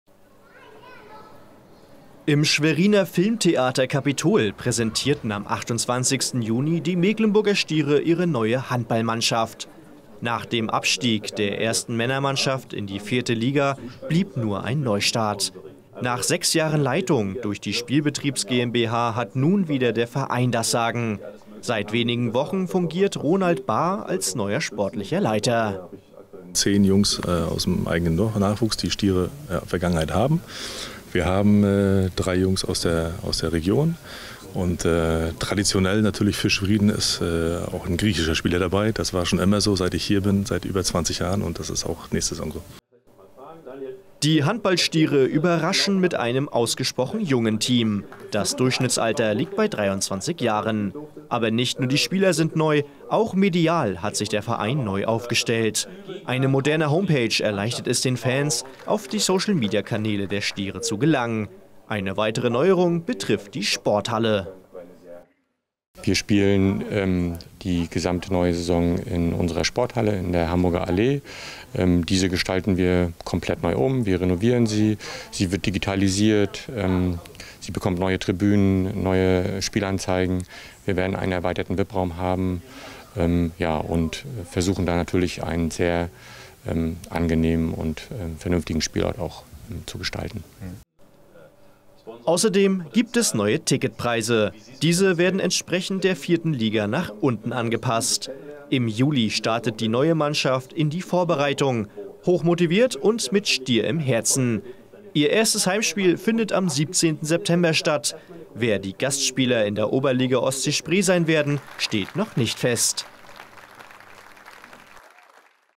Mecklenburger Stiere im Kino
Am 28. Juni stellte der Handballverein Mecklenburger Stiere e.V. sein neues Team vor. Im gut gefüllten Schweriner Filmtheater Capitol konnten interessierte Handballfreunde den sportlichen Leiter Ronald Bahr und Geschäftsstellenleiter Mathias Wächter kennenlernen. Sportliche und mediale Neuigkeiten und Pläne für die neue Saison wurden vorgestellt.